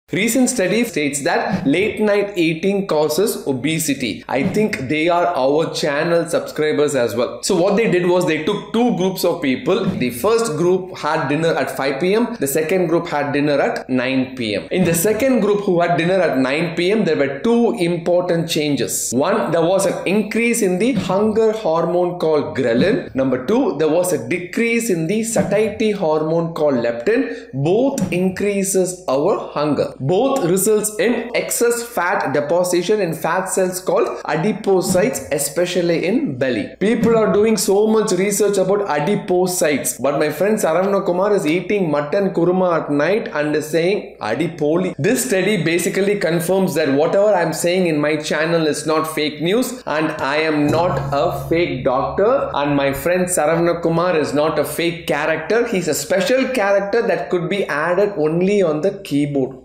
How late-night eating causes Belly fat and Obesity? | Dr Pal
A new study by Harvard Medical School investigators at Brigham and Women’s Hospital found that when we eat significantly impacts our energy expenditure, appetite, and molecular pathways in adipose tissue. They found that eating four hours later makes a significant difference in our hunger levels, the way we burn calories after we eat, and the way we store fat.

Vujović, Scheer, and their team studied 16 patients with a body mass index in the overweight or obese range. Each participant completed two laboratory protocols: one with a strictly scheduled early meal schedule, and the other with the same meals, each scheduled about four hours later in the day.

Results revealed that eating later had profound effects on hunger and the appetite-regulating hormones leptin and ghrelin, which influence our drive to eat. Specifically, levels of the hormone leptin, which signals satiety, were decreased across the 24 hours in the late eating conditions compared to the early eating conditions.

When participants ate later, they also burned calories at a slower rate and exhibited adipose tissue gene expression toward increased adipogenesis and decreased lipolysis, which promote fat growth. Notably, these findings convey converging physiological and molecular mechanisms underlying the correlation between late eating and increased obesity risk.

Bank: Indian bank, Thirunagar branch, Madurai
Account name: AISHWARYAM TRUST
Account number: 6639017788
IFSC: IDIB000T032